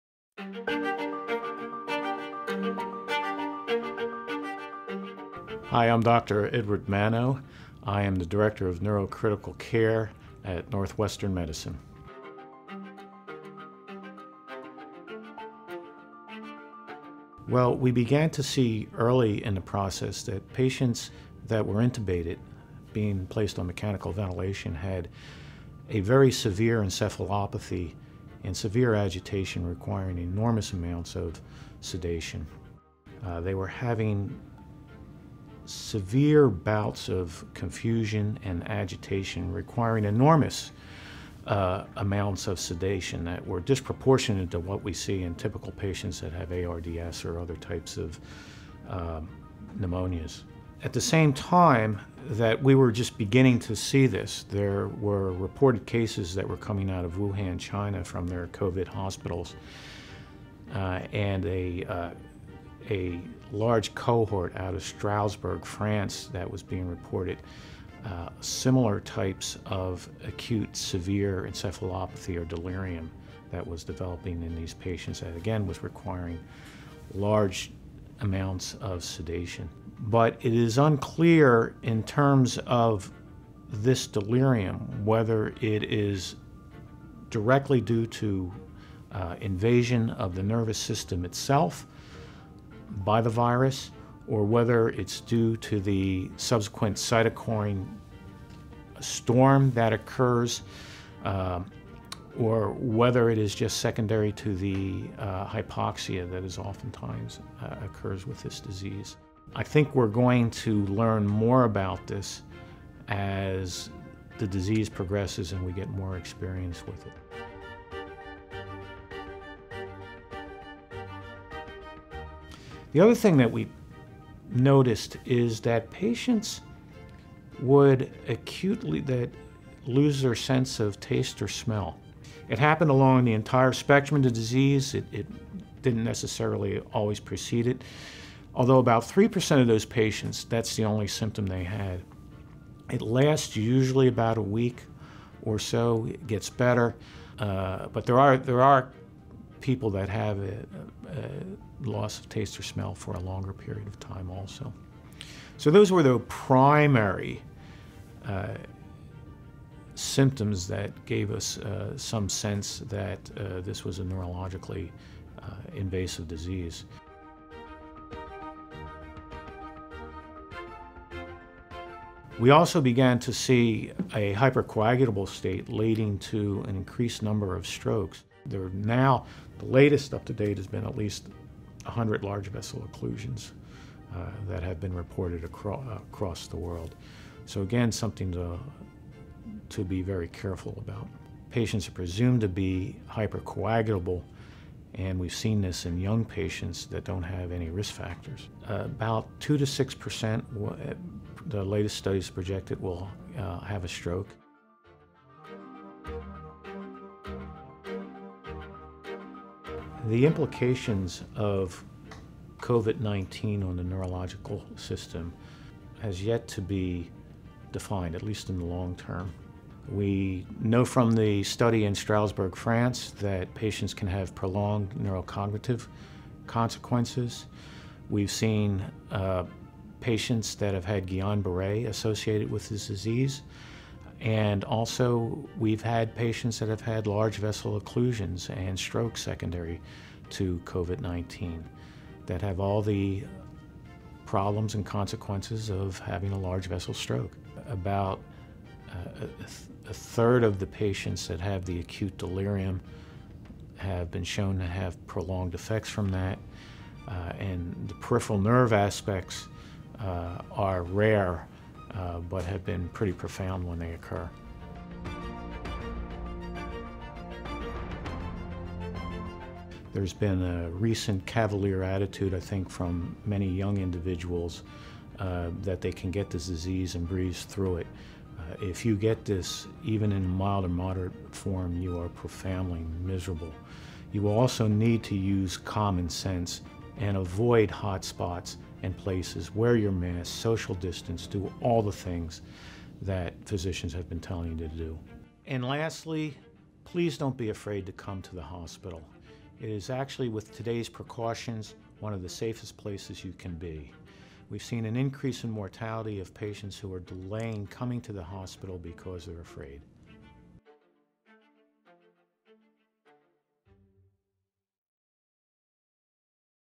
Edward Manno, MD, Summarizes Emerging Evidence of Neurological Manifestations of COVID-19
Edward Manno, MD, MS, professor of Neurology and Neurological Surgery, provides an overview of recent studies and reports of neurological manifestations of COVID-19.